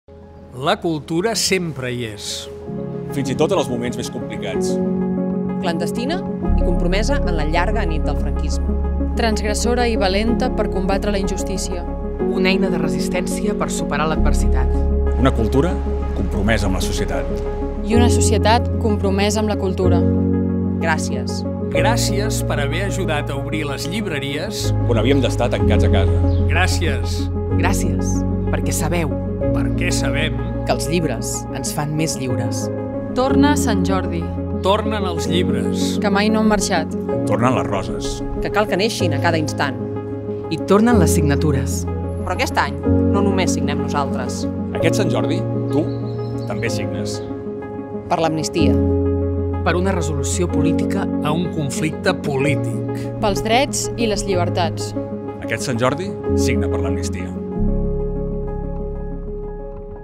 ✍️ [AQUEST SANT JORDI, TU TAMBÉ SIGNES!] ✍️La CULTURA mpre ha estat un espai de RESISTÈNCIA i
✍️ [AQUEST SANT JORDI, TU TAMBÉ SIGNES!] ✍️La CULTURA
sempre ha estat un espai de RESISTÈNCIA i transformació, de lluita pels DRETS i les LLIBERTATS del nostre país
🌹 Aquest Sant Jordi, vine a l'Espai Òmnium i signa per l'Amnistia!
📍Divendres a partir de les 10 h, davant de la Seu Nacional (Carrer de la Diputació, 276, Barcelona)